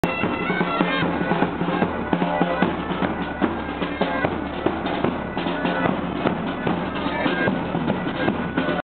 Indian Wedding March
Bride and groom marching into the wedding in India